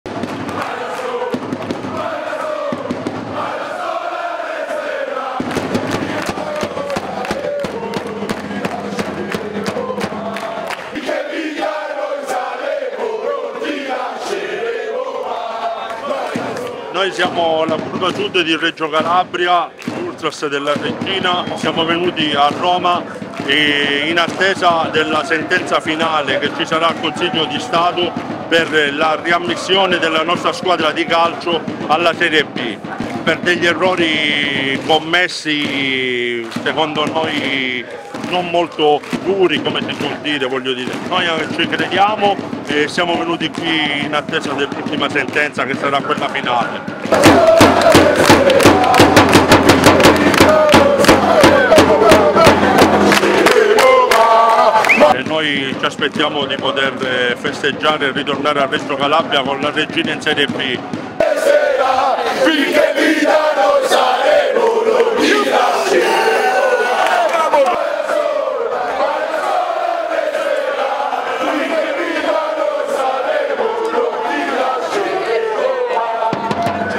«Giustizia per la Reggina» gli ultras calabresi in piazza a Roma
In Piazza Santi Apostoli, a Roma, è andato in scena il sit-in dei tifosi della Reggina in concomitanza con l’udienza del Consiglio di Stato per l’ammissione della squadra in serie B.